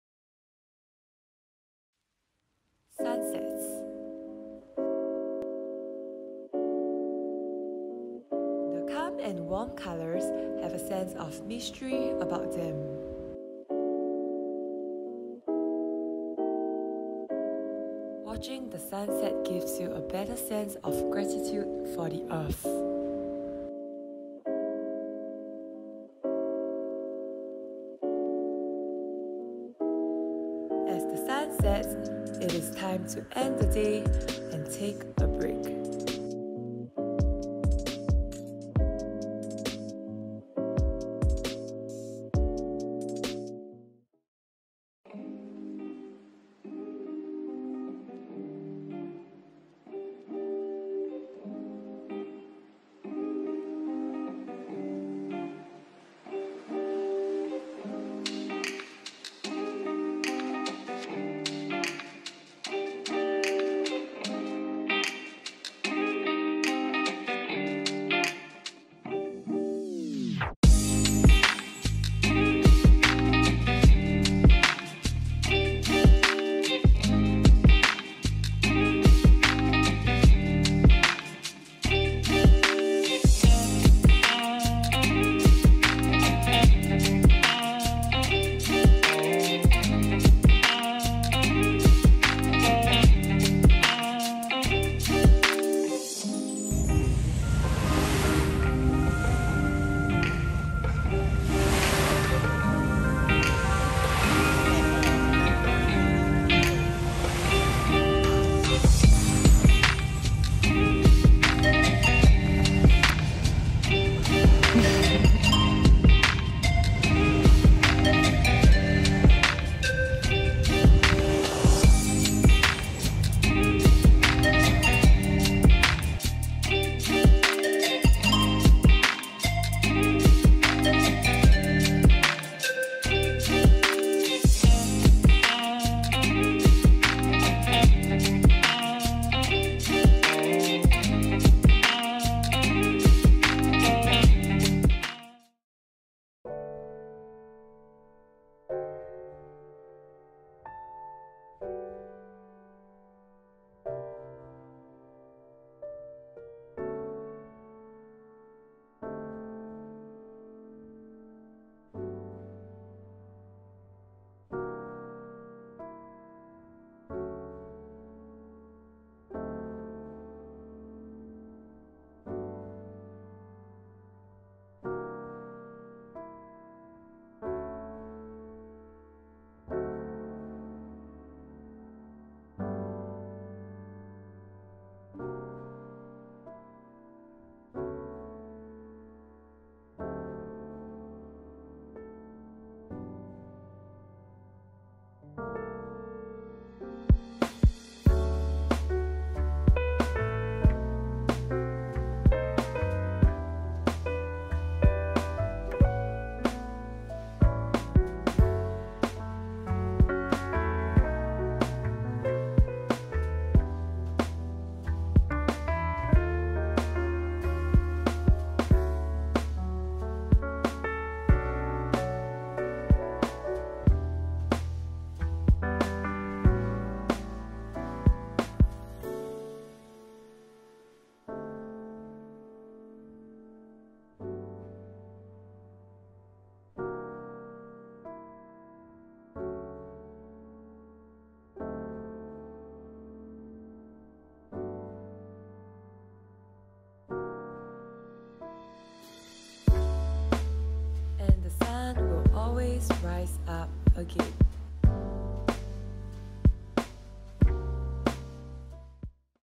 chasing sunsets [a day in my life vlog: beach, cable car, rooftop restaurant]
0:00- sunsets
0:46- sentosa, cable car, beach
2:26- loft
4:08- conclusion

Location: Sentosa, Dusk (restaurant &  bar)